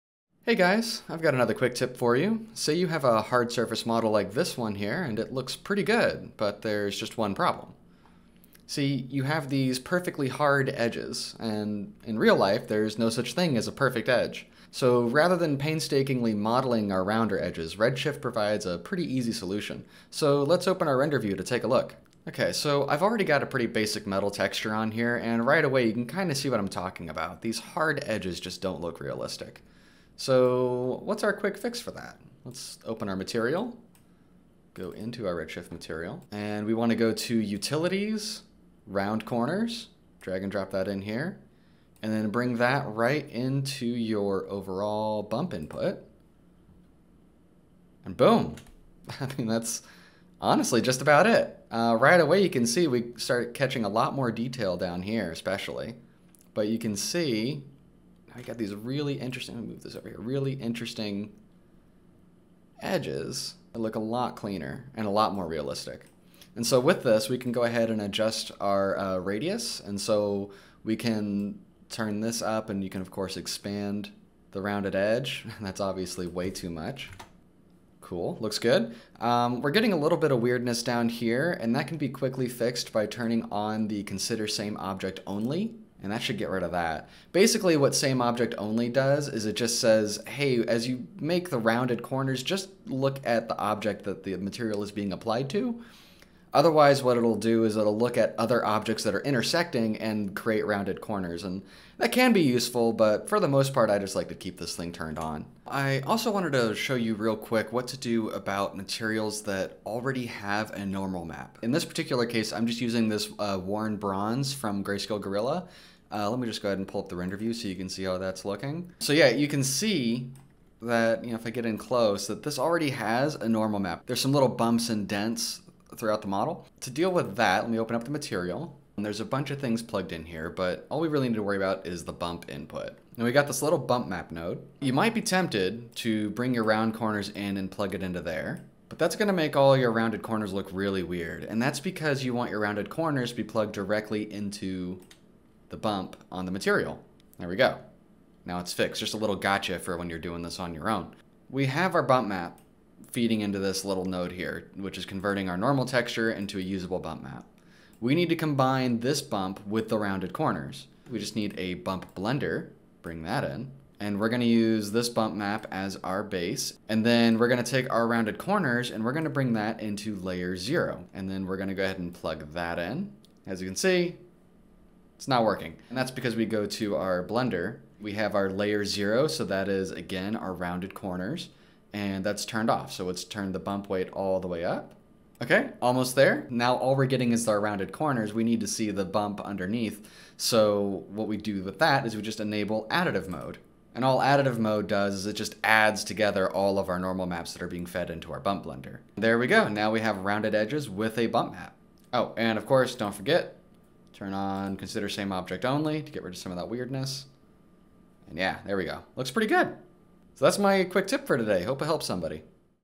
C4D Quicktips: Rounded Edges in Redshift
In this tutorial we look at a really quick and easy way to get rounded edges in Redshift and Cinema 4D. No additional modeling needed!

Chapters:
0:00 Introduction
0:35 Apply Round Corners
1:06 Adjust Round Corner Settings
1:45 Combine with other Normal Maps
2:13 Round Corners Gotcha
2:31 Bump Blender Node